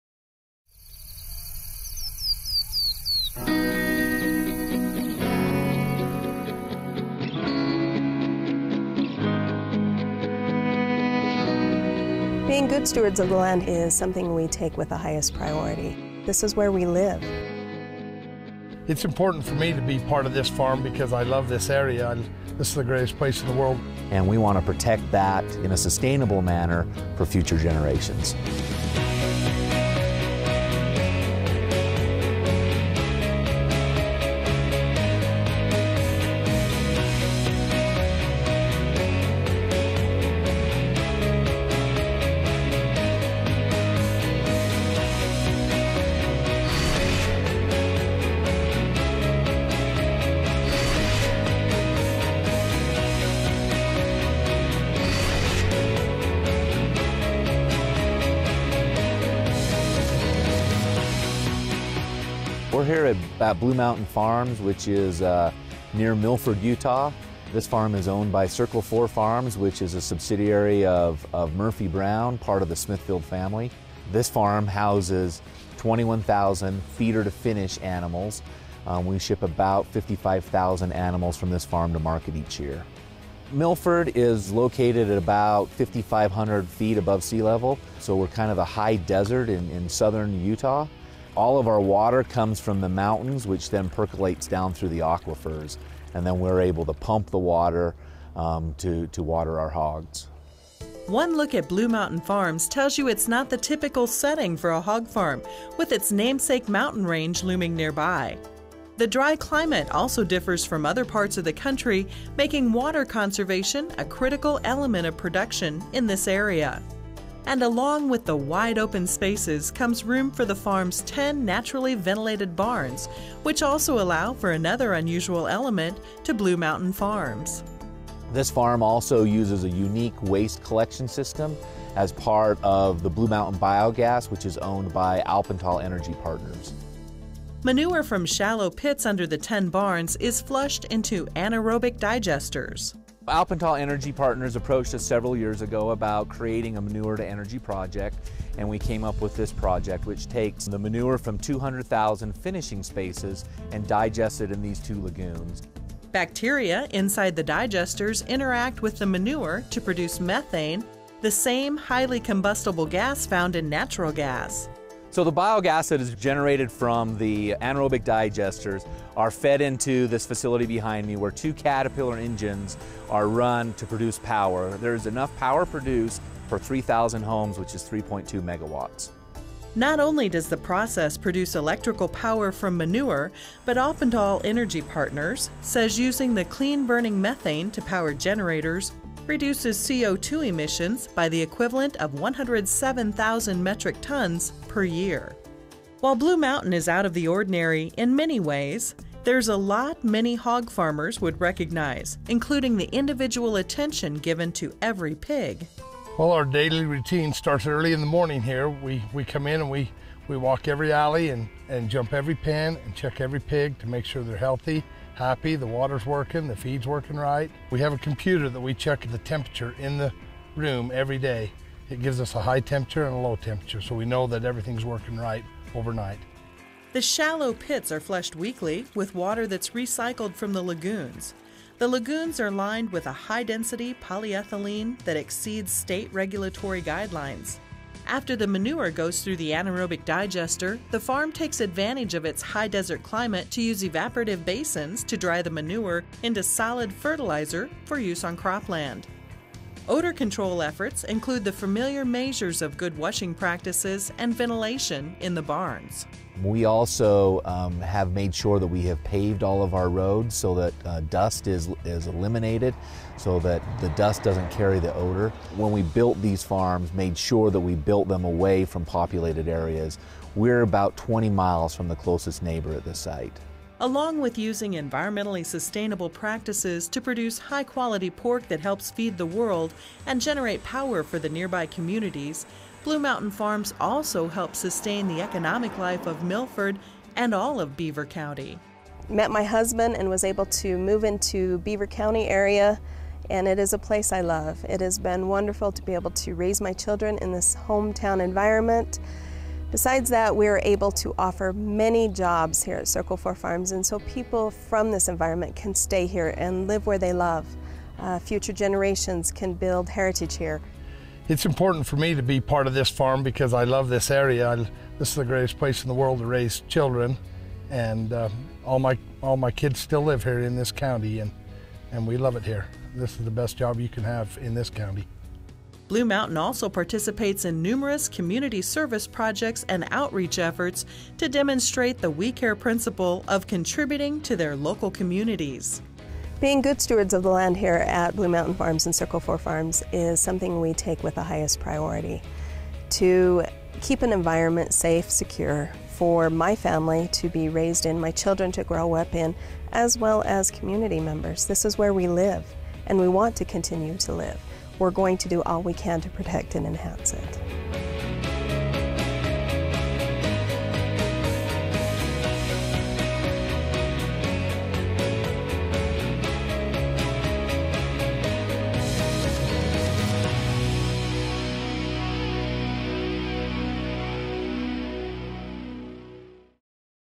2013 Environmental Stewards - Blue Mountain Farm, Milford, Utah
Blue Mountain site 42304, located in the high desert of southern Utah, produces 55,000 hogs annually in 10 feeder-to-finish barns. The farm, a Murphy-Brown subsidiary and offshoot of Smithfield Foods, takes steps to minimize its environmental footprint and give back to the community, says Jim Webb, environmental, safety and public affairs manager for Circle Four. Blue Mountain farm supplies manure to Alpental Energy Partners, which uses anaerobic digesters and large Cat engines to produce electricity.